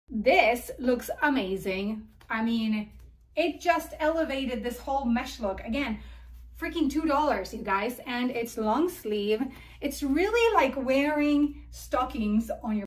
$2 SHEIN Mesh Bodysuit is a Must Have!
You already know SHEIN is the spot for amazing deals!  These mesh bodysuits were so worth it!

Want to send something? Go ahead! I love mail!